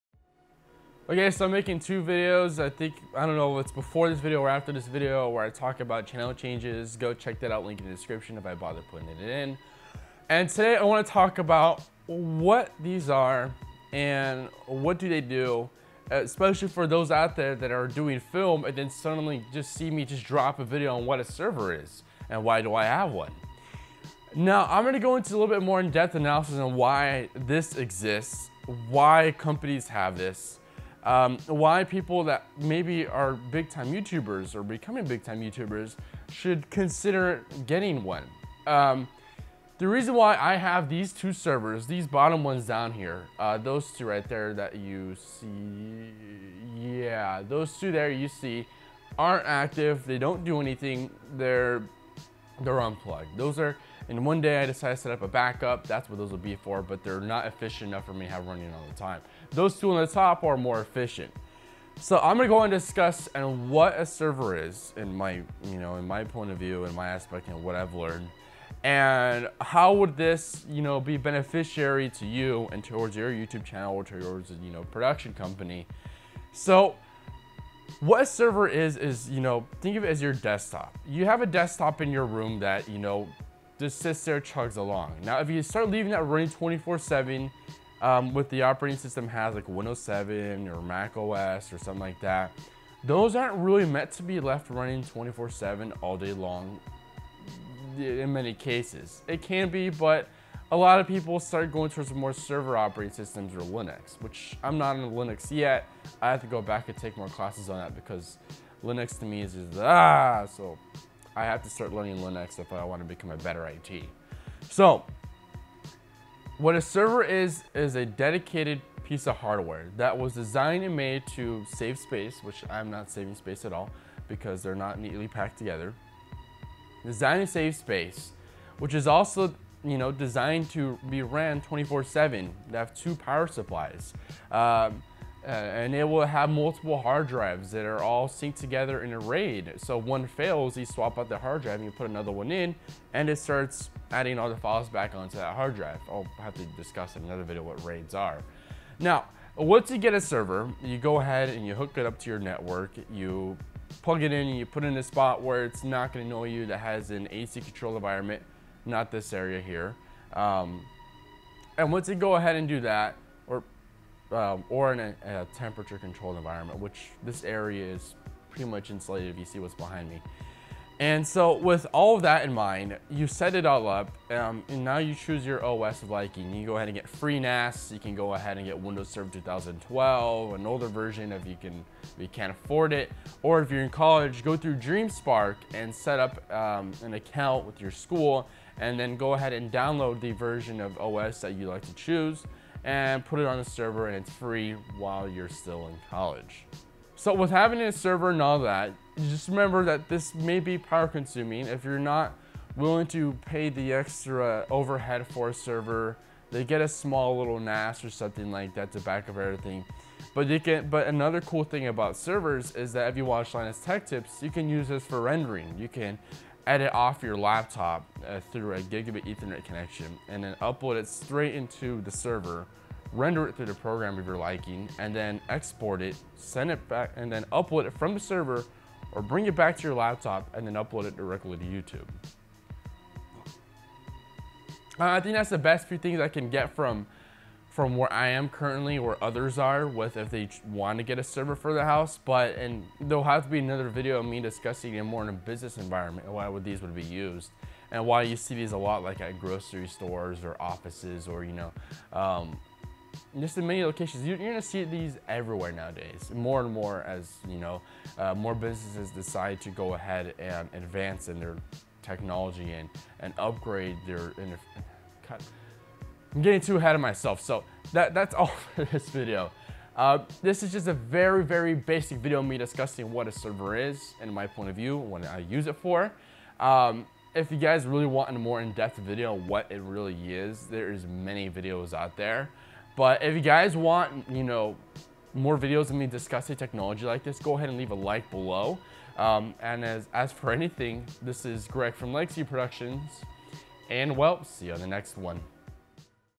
What are Servers?
Now I am not like, Eli The Computer Guy or like Linus Tech Tips. I will be talking about what servers are and how they can benefit you.